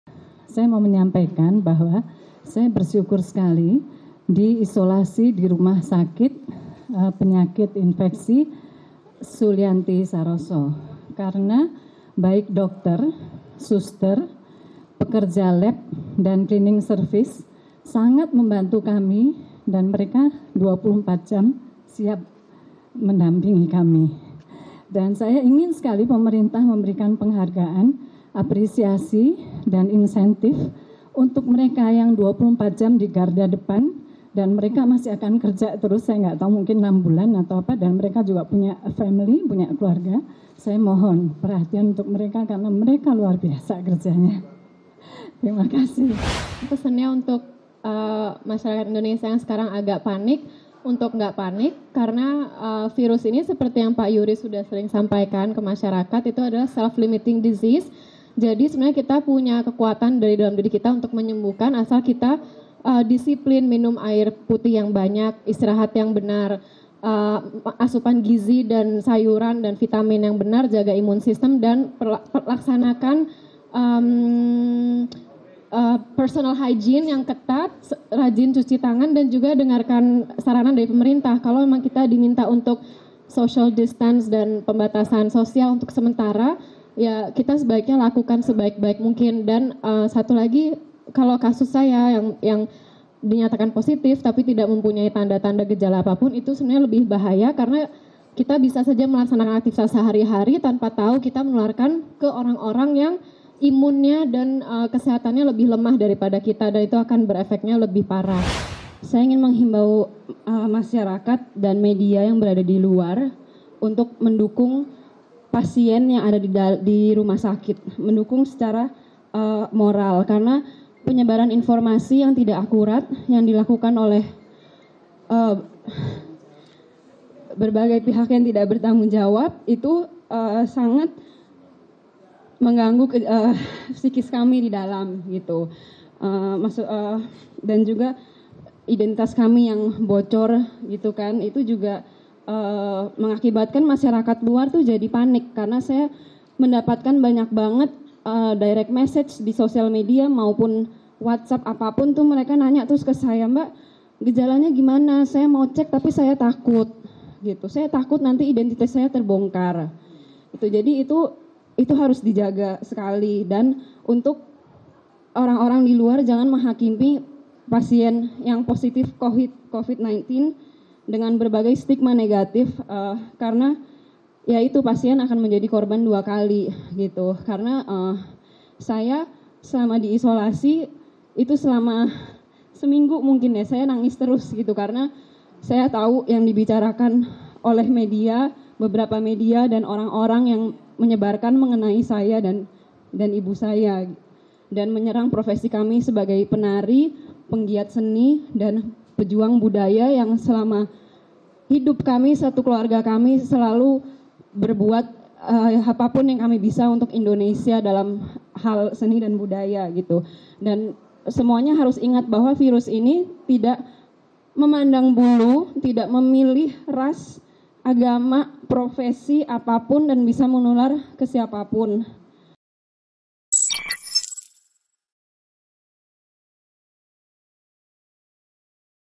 Pernyataan dari 3 Pasien Positif Korona yang Dinyatakan Sembuh
Pasien positif covid-19 nomor 01, 02, dan 03 dinyatakan sembuh dan diperbolehkan kembali ke rumah. Sebelum kembali mereka mendapat bekap jamu ramuan Presiden Jokowi untuk menjaga kesehatan di rumah.